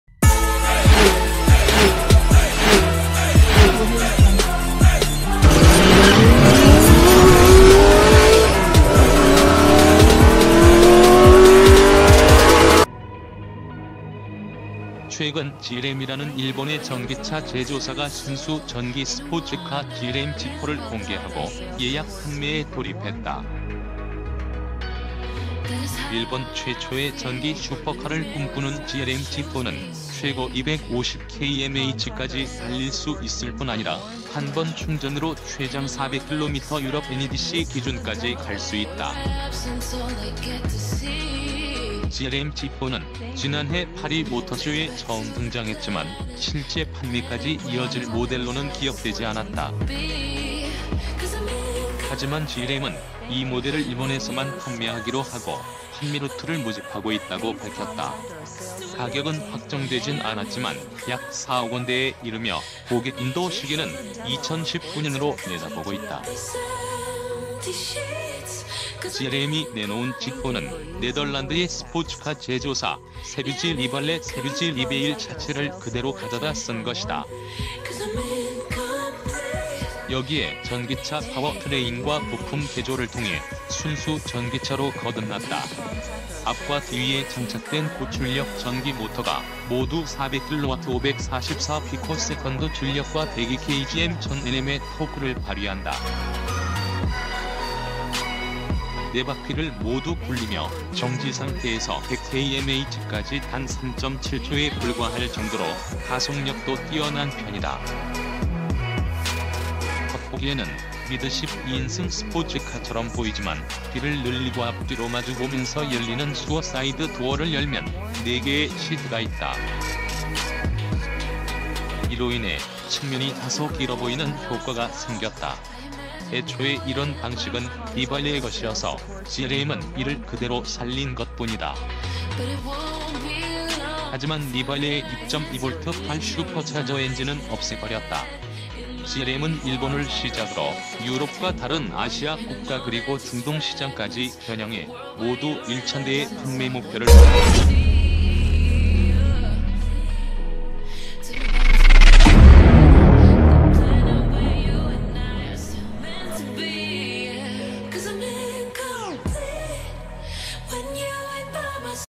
GLM G4, 일본 최초 전기 슈퍼카를 꿈꾸다
,4인승,GLM/G4,
GLM G4, 일본 최초 전기 슈퍼카를 꿈꾸다 ► Subscribe for more 최근 GLM이라는 일본의 전기차 제조사가 순수 전기 스포츠카 ‘GLM G4’를 공개하고 예약 판매에 돌입했다. 일본 최초의 전기 슈퍼카를 꿈꾸는 GLM G4는 최고 250kmh까지